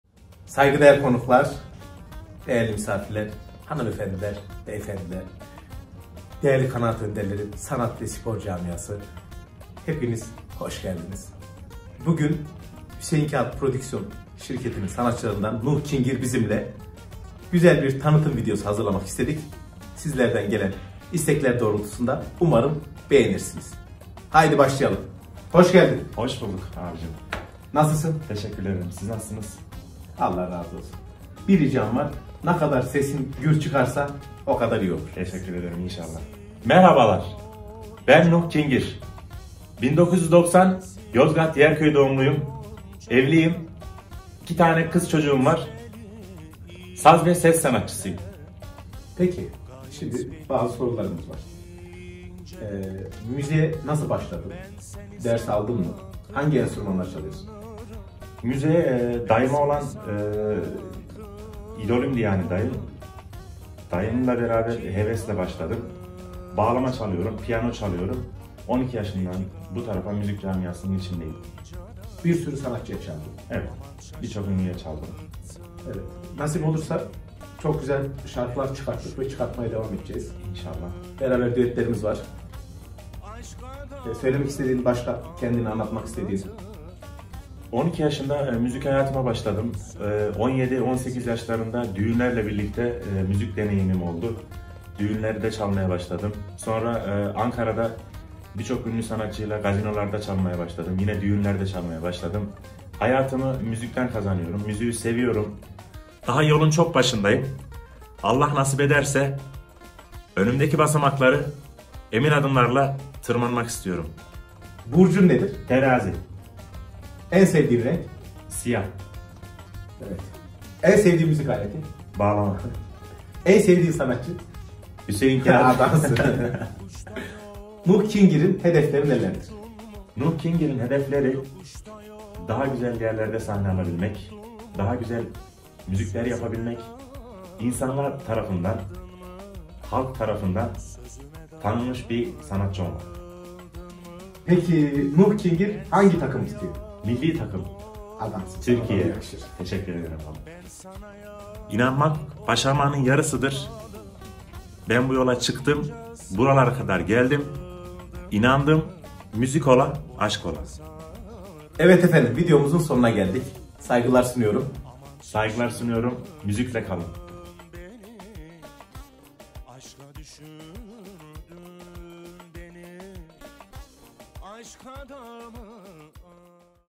Hüseyin Kağıt & Nuh Kingir - Sohbet Vlog Video
Hüseyin Kağıt Prodüksiyon Sanatçılarından
Nuh Kingir Kimdir ?
Hüseyin Kağıt Soruyor !!!